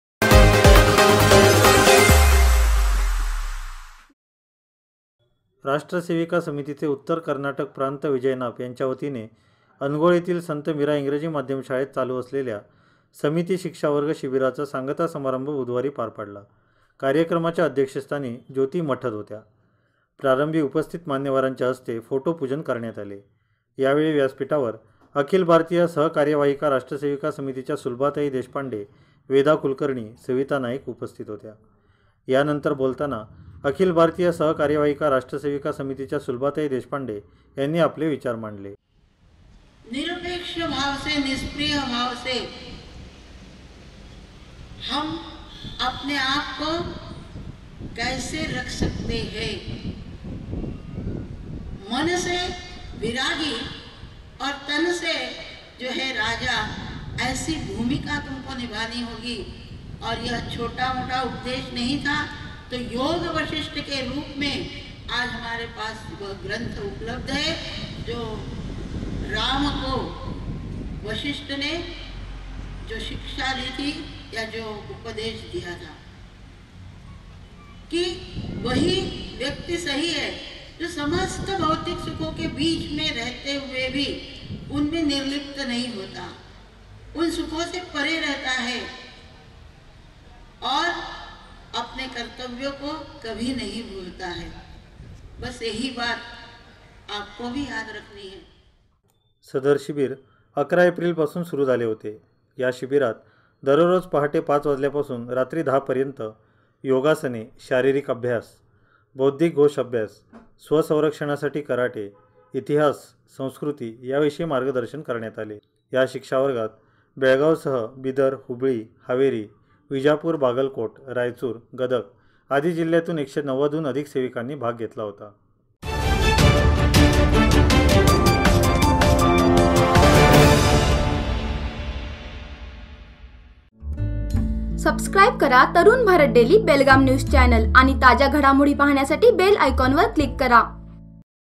राष्ट्रसेविका समितीच्या शिबिराची सांगता-Belgaum News
|तरुण भारत| प्रारंभी मान्यवरांच्याहस्ते करण्यात आले फोटो पूजन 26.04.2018